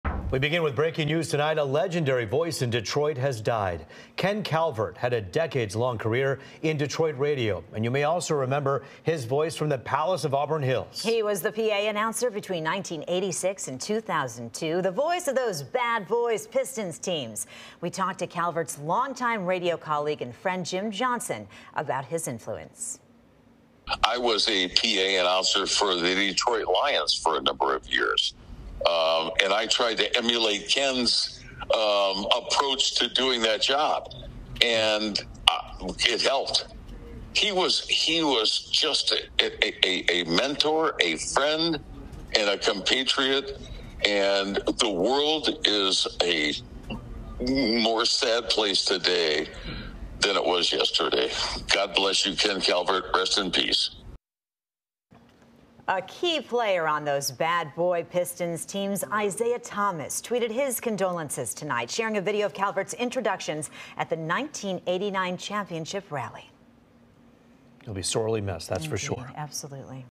Detroit loses beloved radio personality Ken Calvert
Ken Calvert worked for several Detroit rock stations during his decades in the business. He was also a Detroit Pistons PA announcer for 16 years.